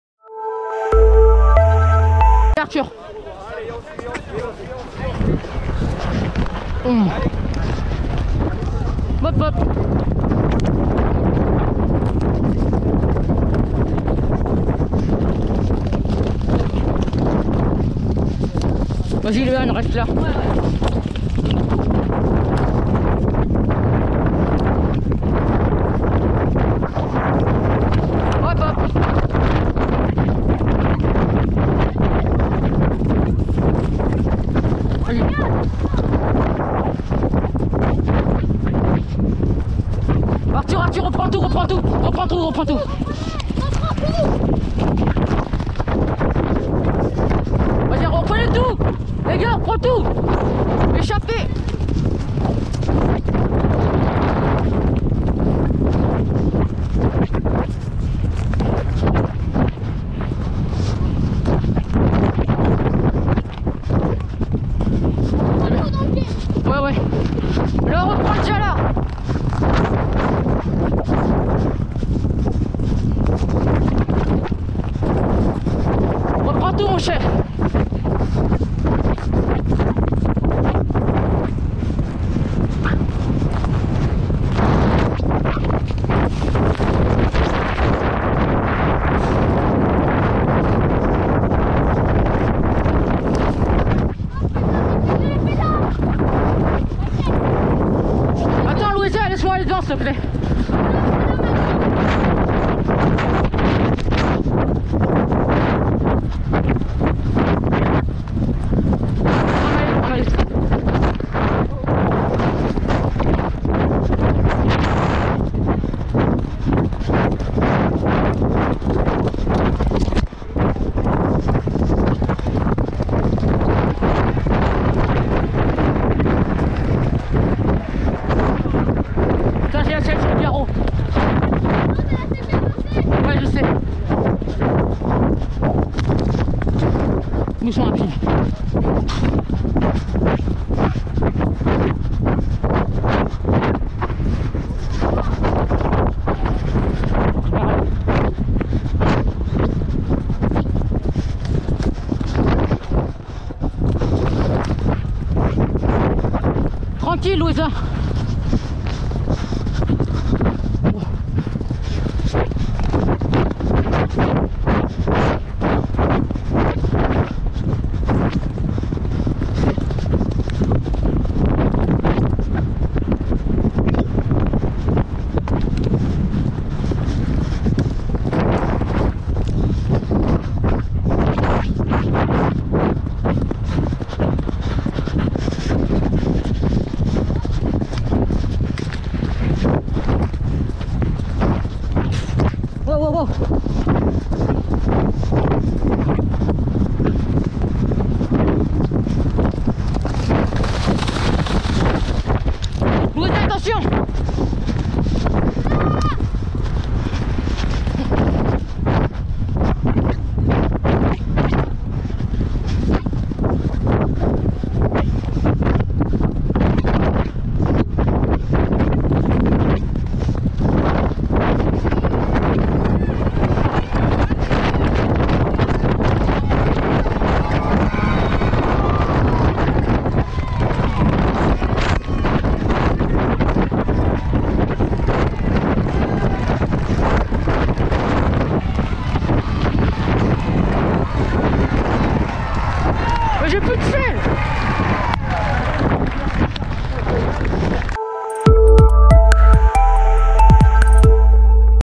France Sire Poneys Cross Challenge #8: en selle avec Charles Clayeux à Pompadour
Grâce à la GoPro portée par Charles Clayeux, en selle sur Divine du Mayne, revivez de l'intérieur la 8ème étape du France Sire Poneys Cross Challenge qui s'est disputée ce lundi 15 août sur l'hippodrome de Pompadour (19).